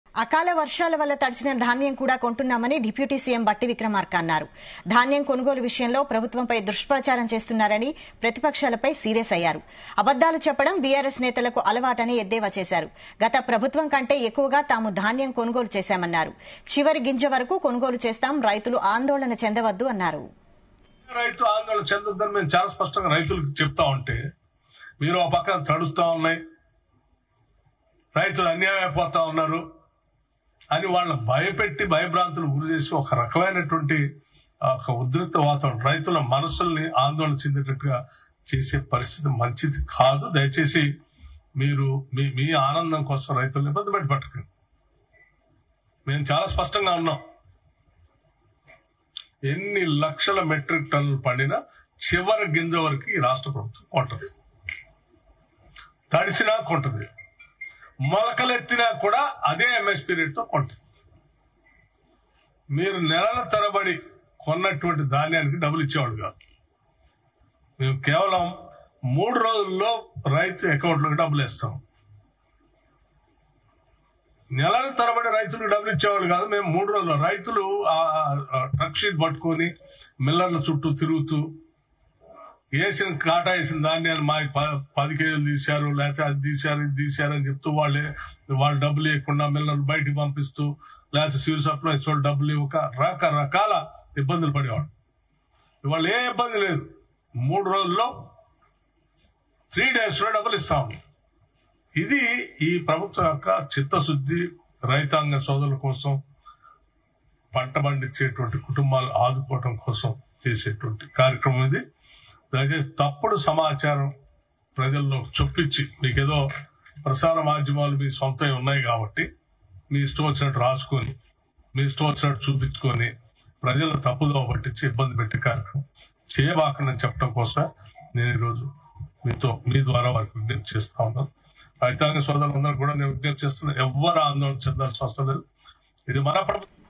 Deputy CM Bhatti Vikramarka Comments on BRS | అబద్ధాలు చెప్పడం బీఆర్ఎస్ నేతలకు అలవాటే!! |ZEE News
అబద్ధాలు చెప్పడం బీఆర్ఎస్ నేతలకు అలవాటే!! | Deputy CM Bhatti Vikramarka Comments on BRS | ZEE Telugu News